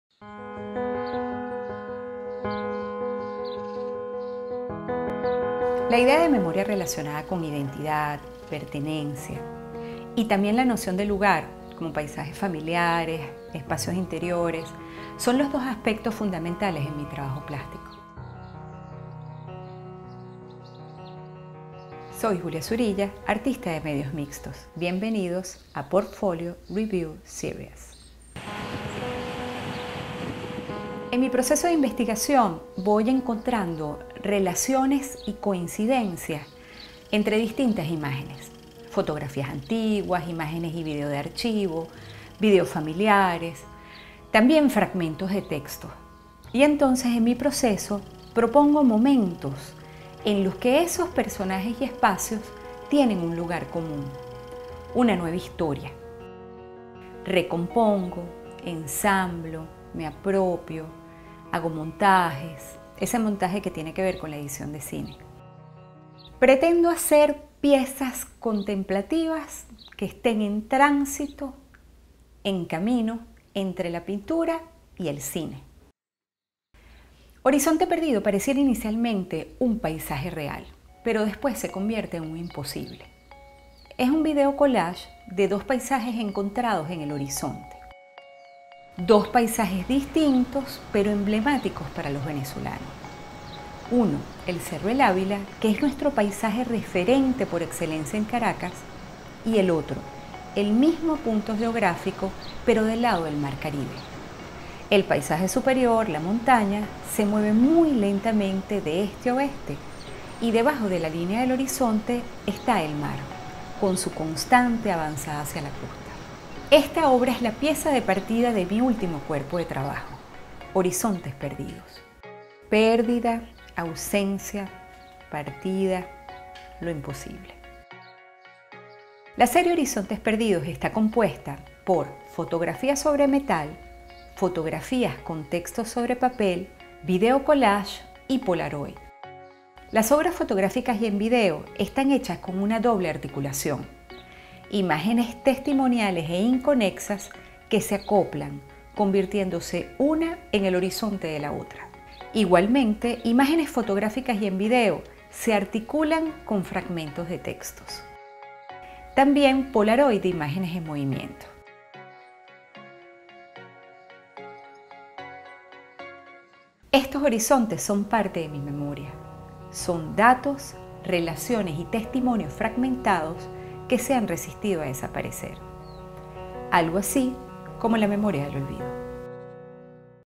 Memorias del olvido Portfolio Reviews Series
Short video about the mix media artist Julia Zurilla's work.
Shooting for The Portfolio Review Series of The Coral Gables Museum.